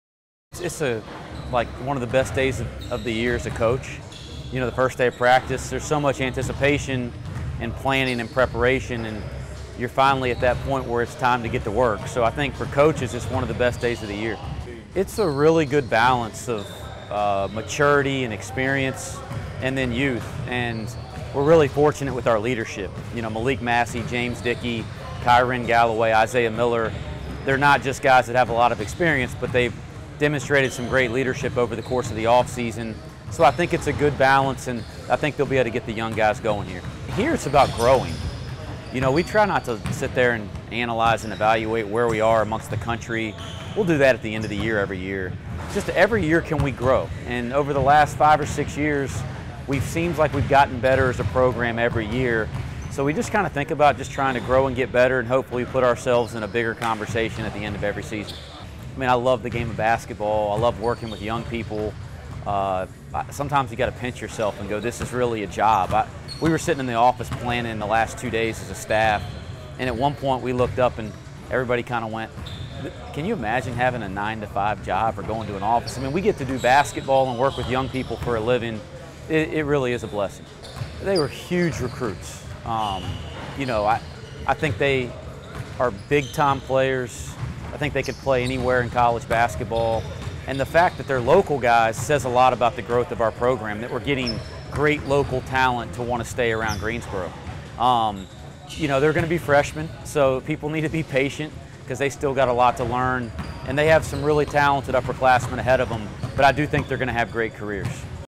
Interview With UNCG Basketball Head Coach Wes Miller
Miller Is Entering His Ninth Season As Head Coach Of The Spartans